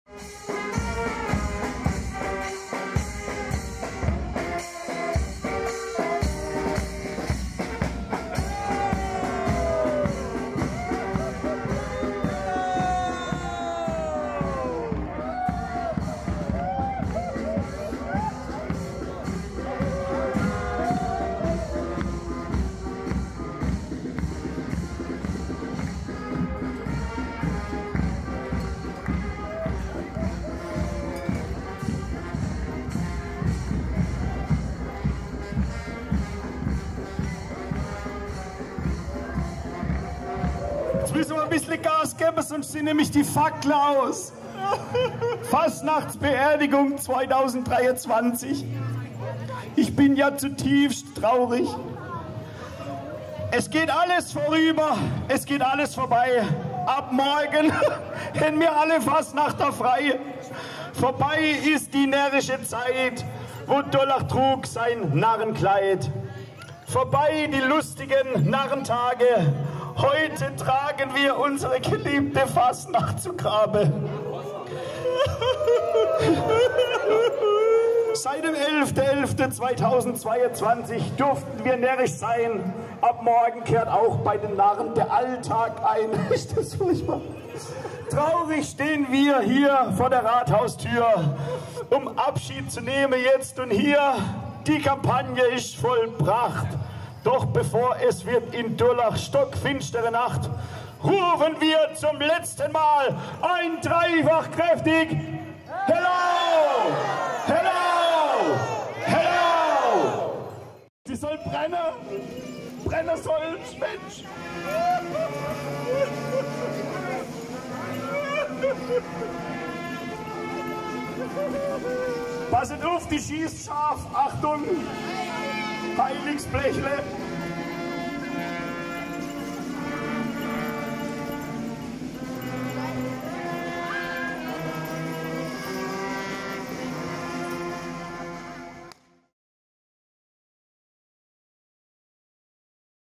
Fastnachtsbeerdigung in Durlach 2023
Seit Jahren wird am Abend des Faschingdienstags in Durlach die Fastnacht anständig beerdigt. Die Narren treffen sich ein letztes Mal, geben widerwillig den Rathausschlüssel zurück und verbrennen die Fastnacht. Aber keine Angst, sie kommen wieder ... keine Frage!